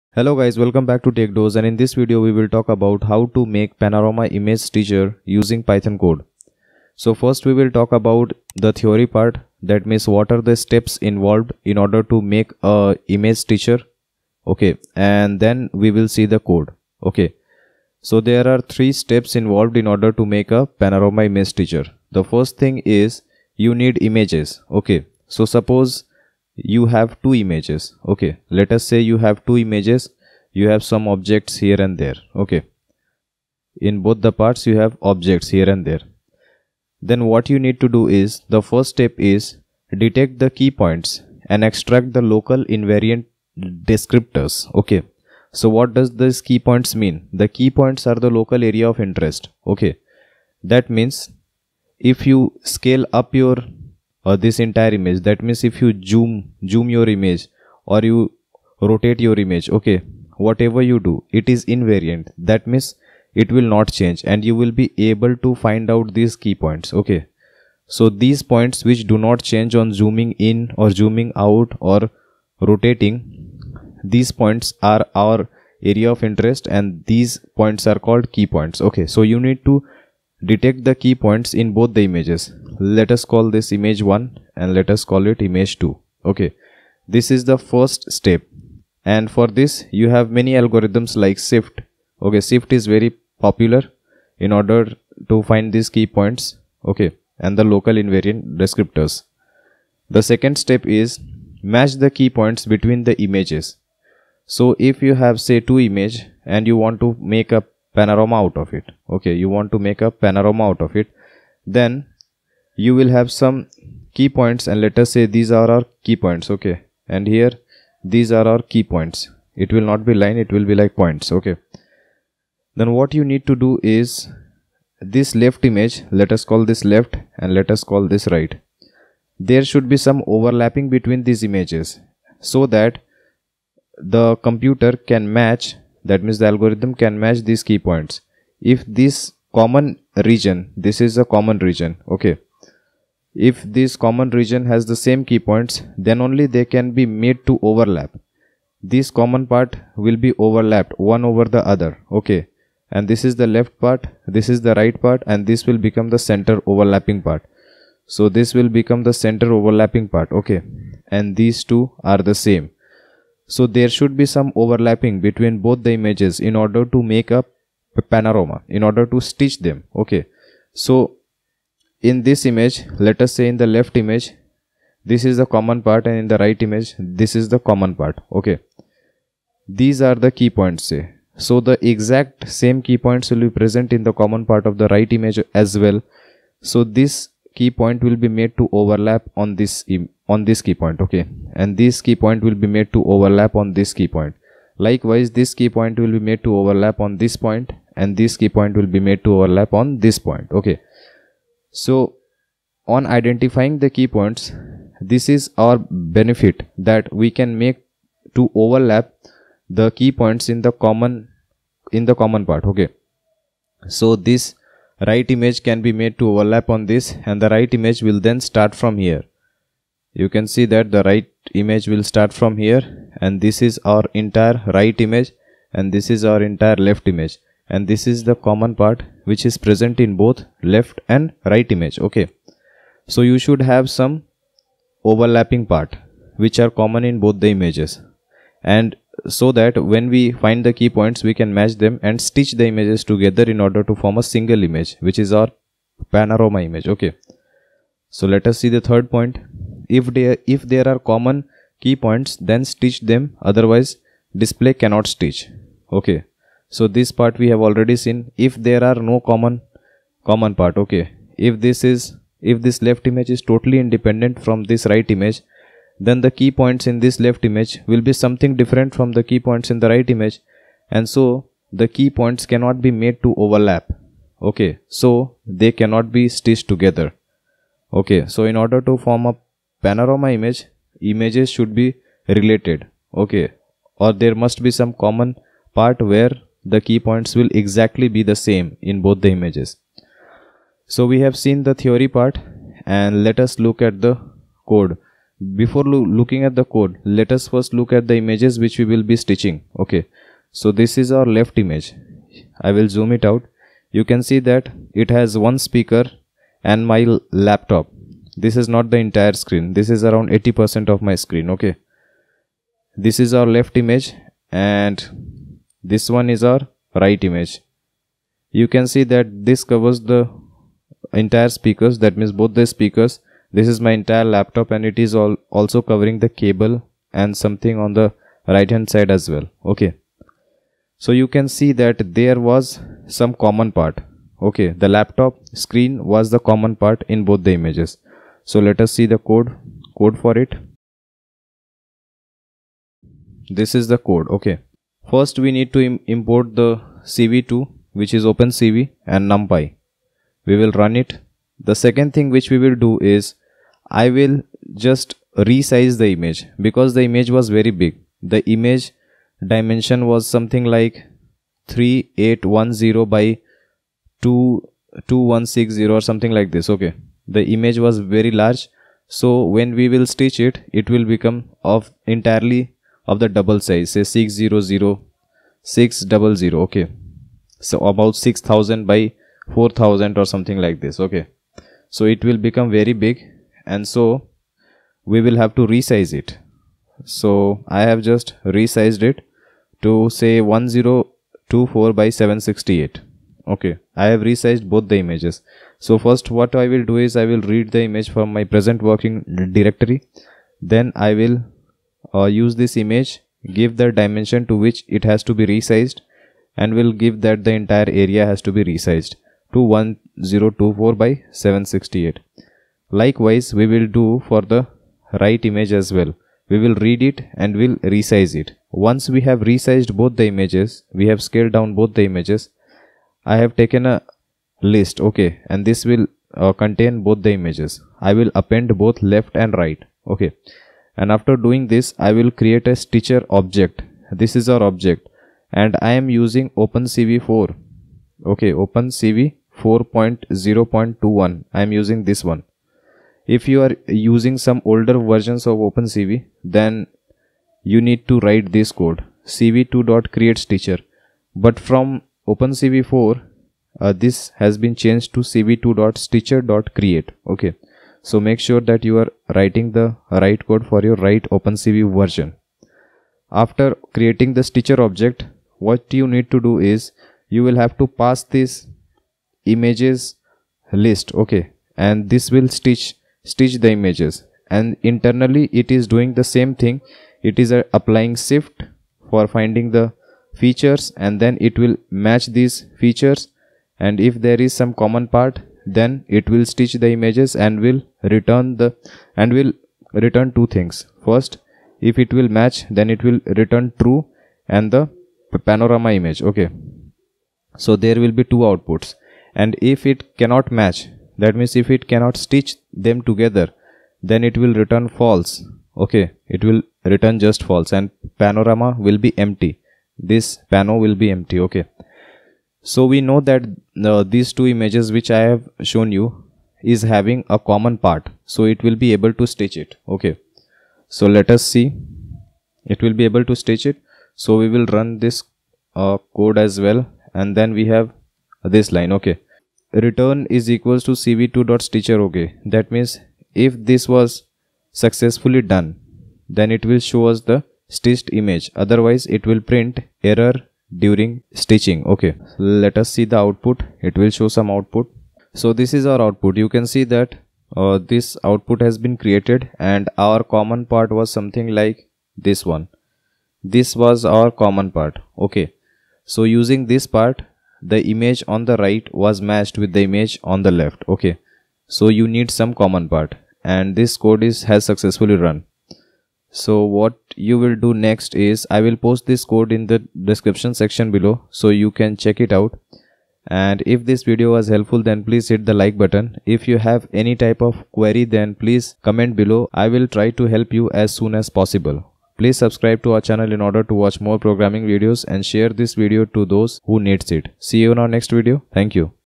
Panorama Image Stitcher (with Python CODE)
This video explains how to stitch images in order to form PANAROMA image. This video explains panaroma image stitching working, process and algorithm with simple to inderstand Python code using OpenCV.